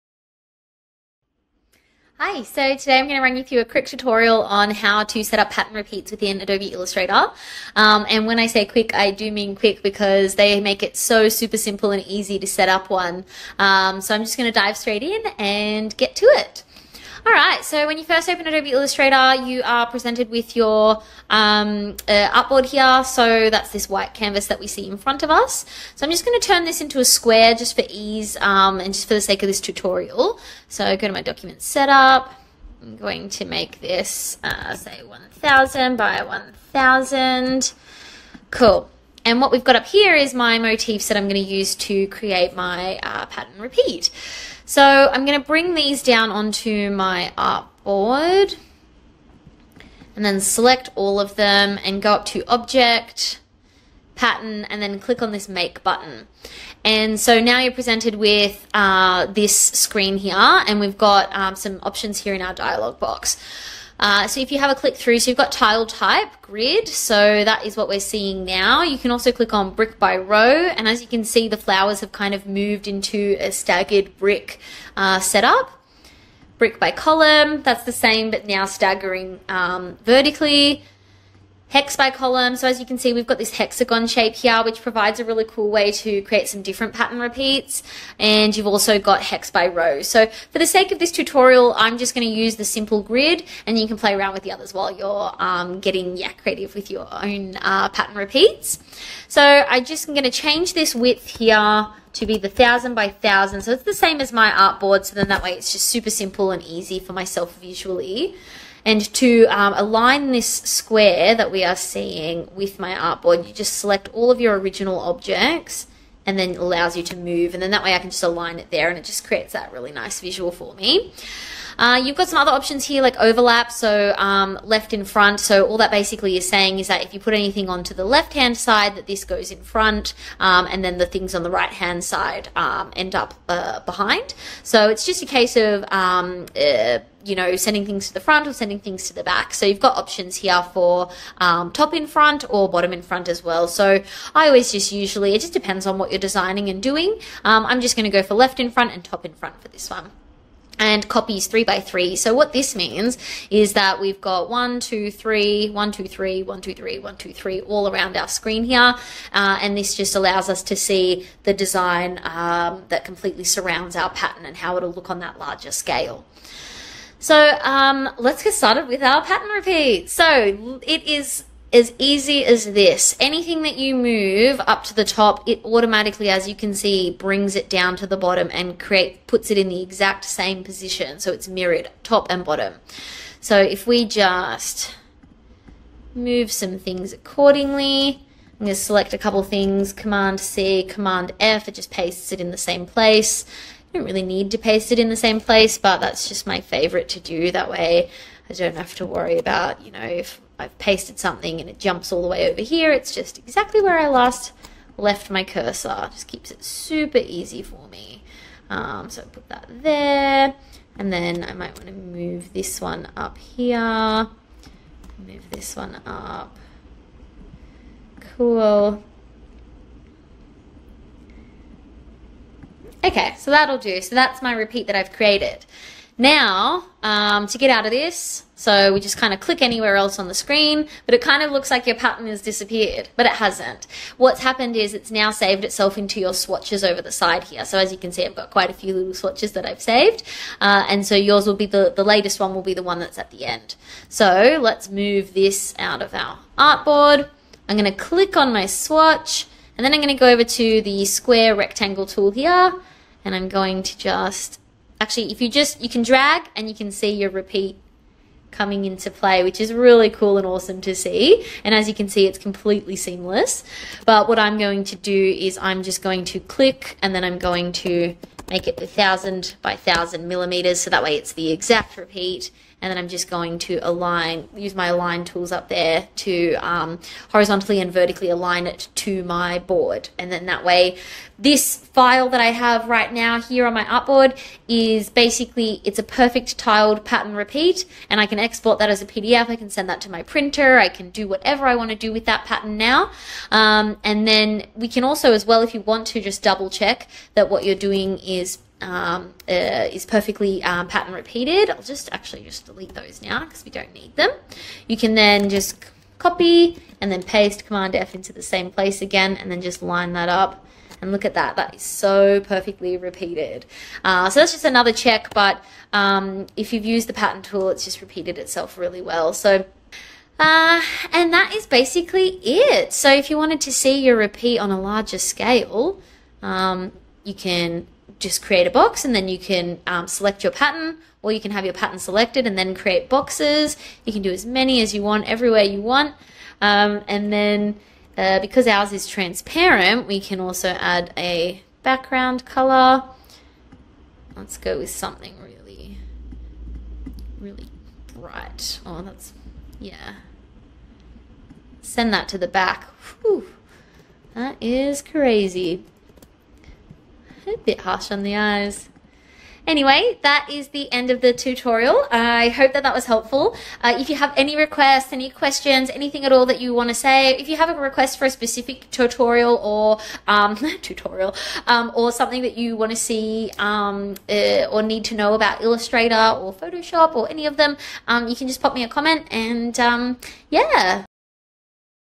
How to create a pattern repeat in Adobe Illustrator
Are you ready to learn the 7 easy steps to create professional seamless repeating patterns in Adobe Illustrator?

Whether you're a novice or a seasoned pro, mastering the art of pattern repeating in Adobe Illustrator is the key to unlocking your full creative potential.

In this tutorial, you'll discover how to effortlessly create seamless repeating patterns and access one of our stunning flower patterns for free.

To follow along you can download the same floral motifs